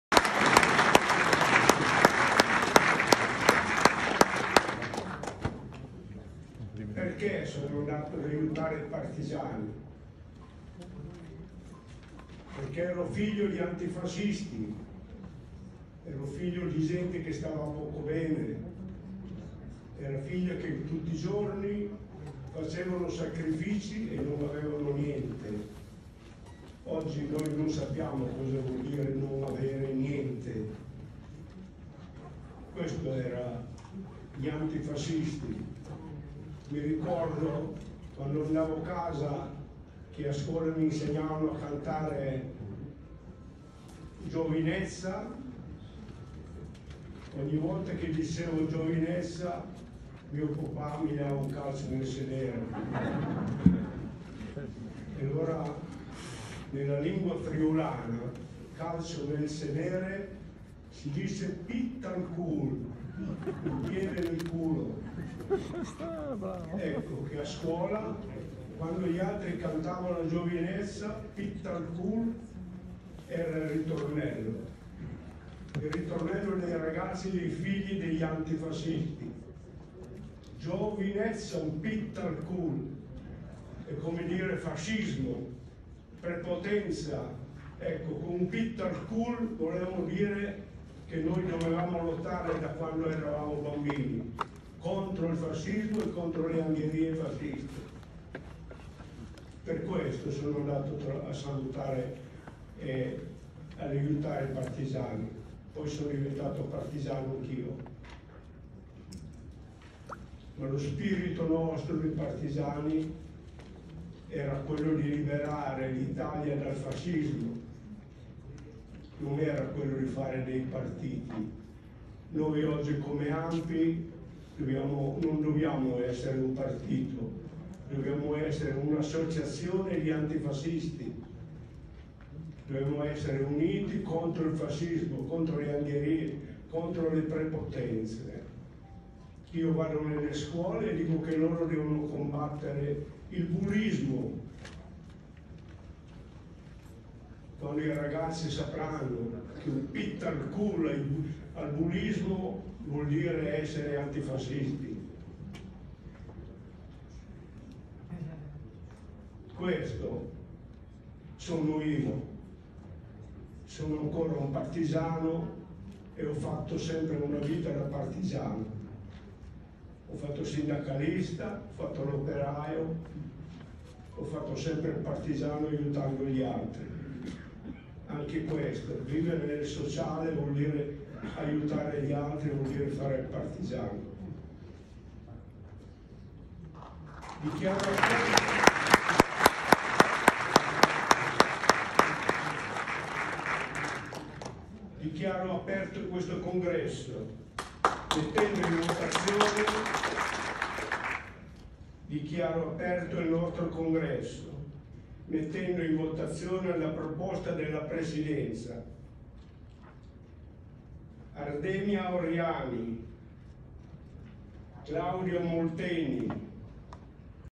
Intervento Giovanni Marzona "Alfa". ANPI Congresso Provinciale Milano
XVII Congresso ANPI Provinciale Milano. 12/13 Marzo 2022. Centro Congressi NH Milanofiori Assago (Mi) - Intervento d'apertura congresso del Partigiano Alfa. Giovanni Marzona